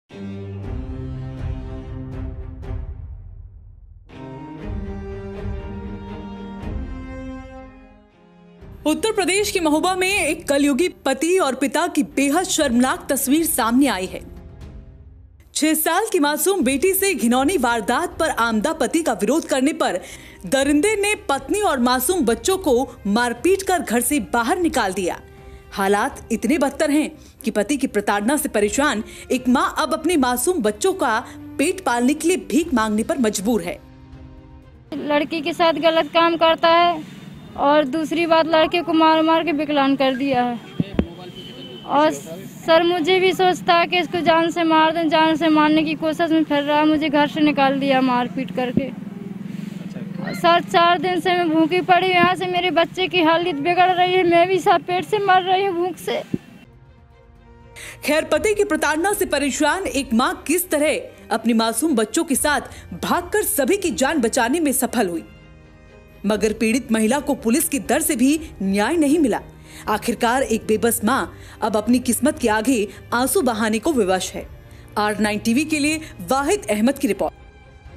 महोबा का महापापी पिता !
पति के गंदे मंसूबों ने पत्नी और बच्चों को किया बेघर। भूखे पेट भीख मांगने को मजबूर हुई एक मां...